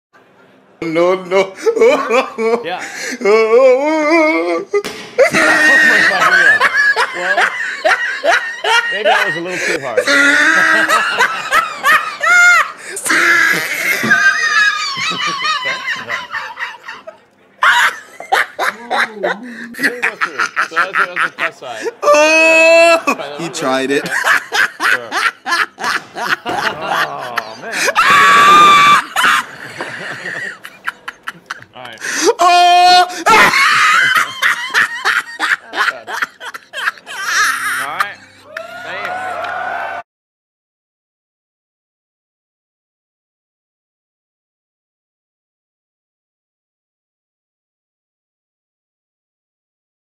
I put this LAUGH over the Cybertruck's Window SMASHING clip
I put a laugh track over the moment where the Cybertruck's window breaks in an on-stage demo and it's even funnier.